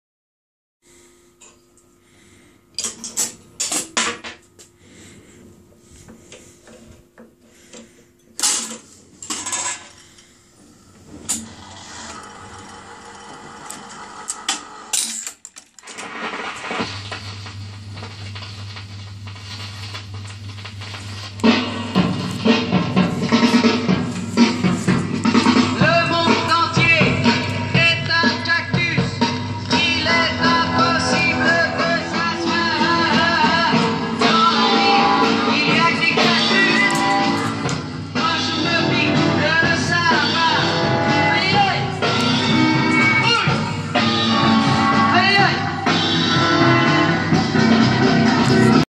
Matebois S80 enfin du son!
Suite à la remise en état de l'ampli, j'ai enfin du son pour profiter pleinement du jukebox :-))